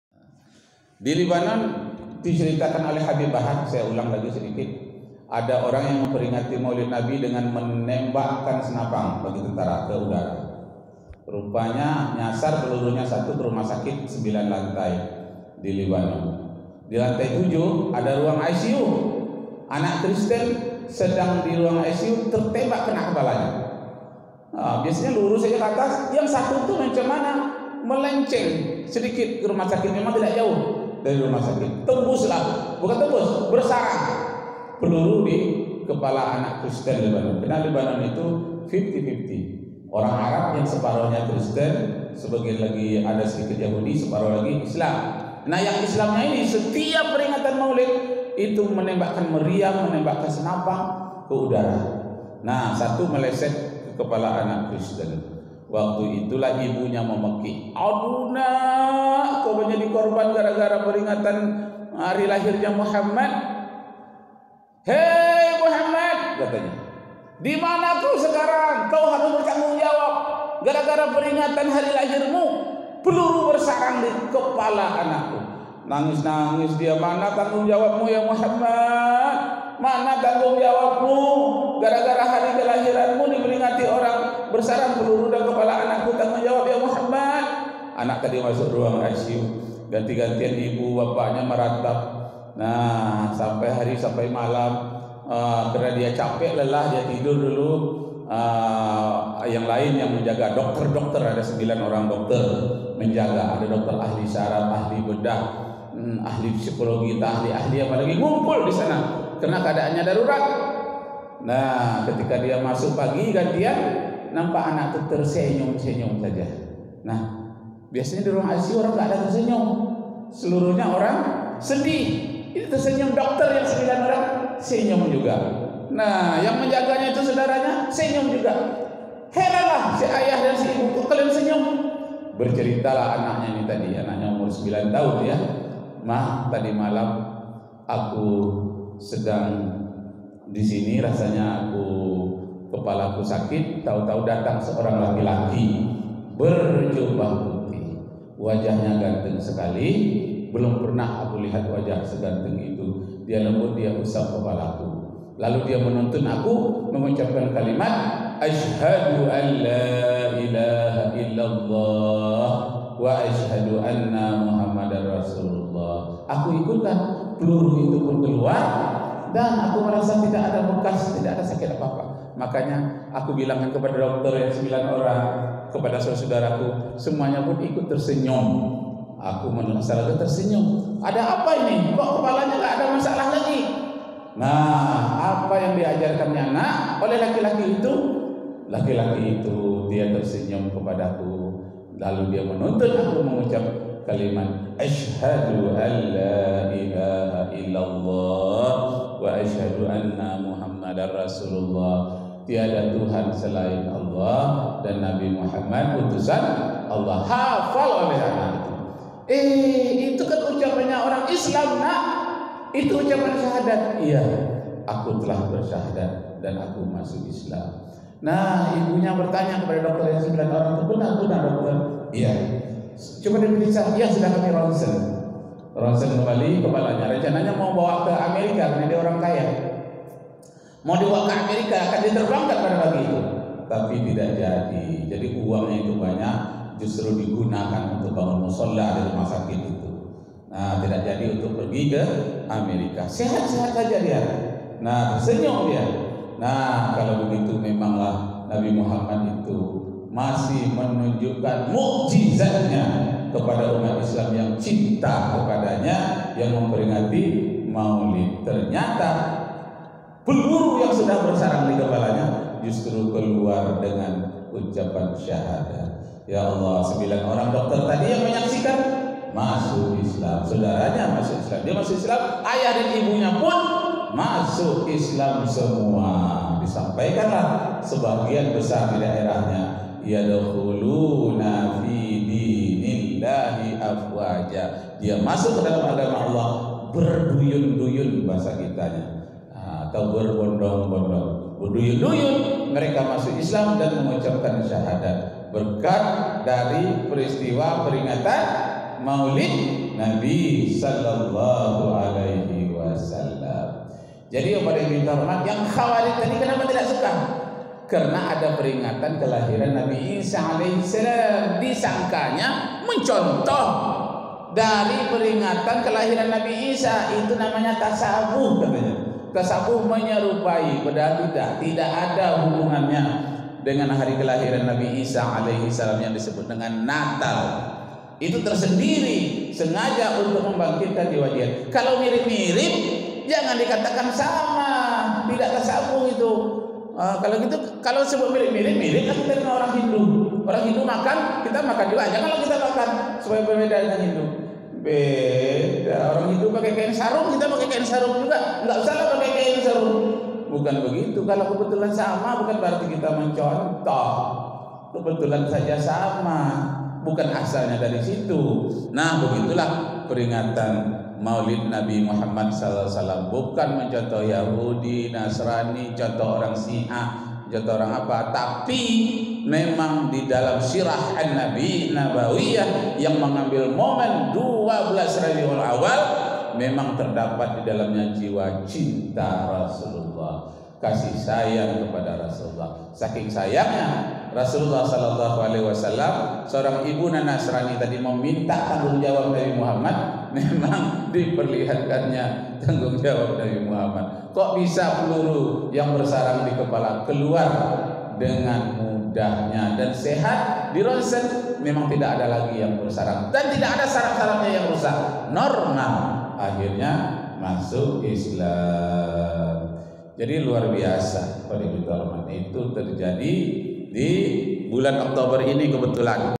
PELURU BERSARANG DI KEPALA, SAAT ACARA MAULID
HABIB BAHAR MENCERITAKAN BAHWA ADA KEJADIAN DI LIBANON, ACARA PERINGATAN MAULID DIRAYAKAN DENGAN TEMBAKAN PELURU KE UDARA, TAPI NYASAR KE  KEPALA SEORANG ANAK DI RUMAH SAKIT.., REKAMAN M,RAKIB JAMARI.